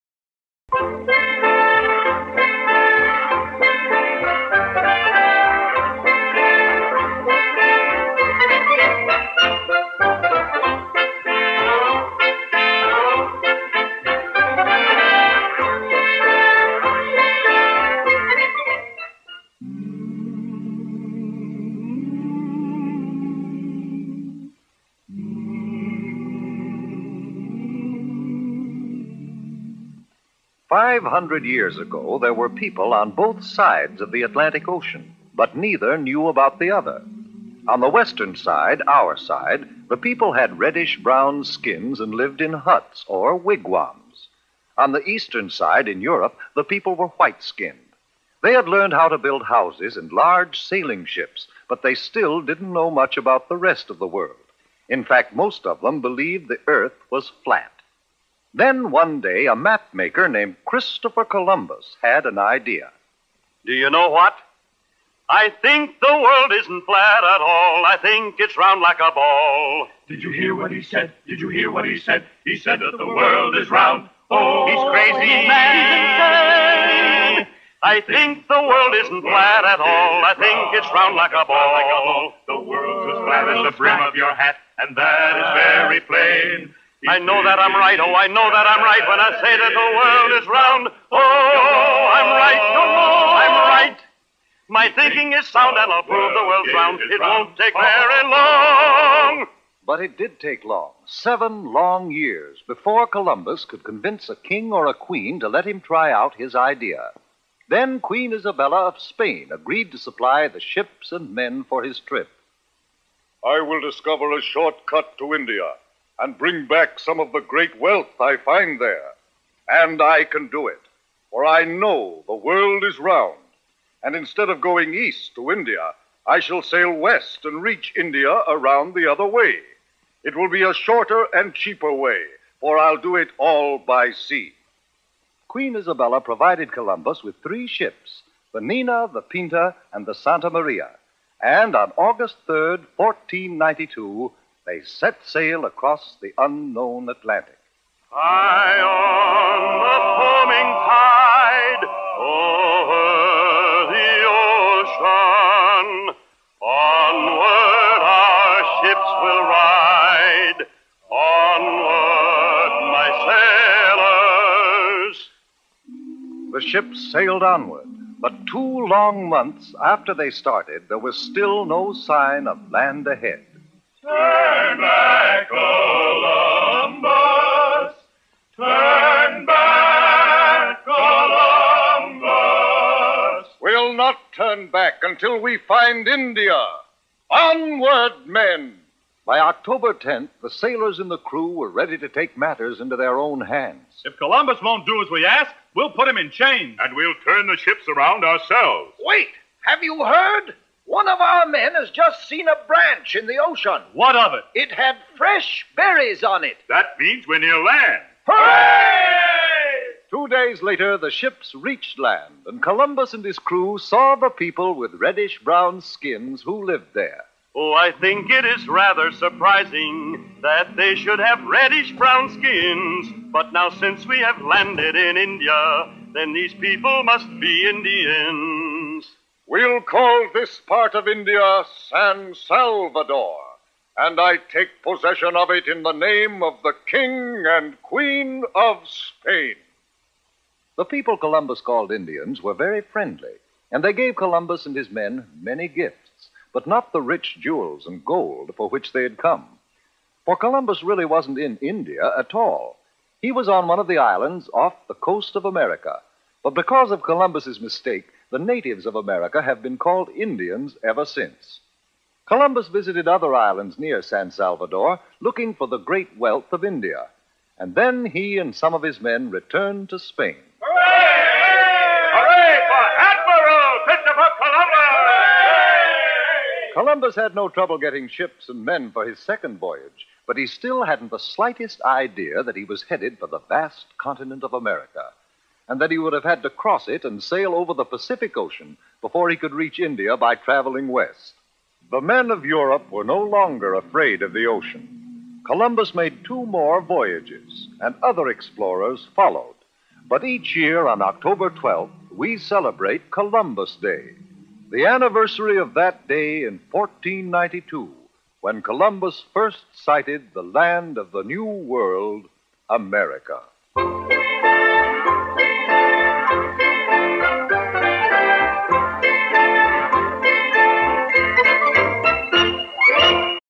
Vintage Columbus Day Video (public domain)
This vintage cartoon about Christopher Columbus used to be shown in schools around Columbus Day.  This is a great example of historical propaganda American school children were and often are exposed to.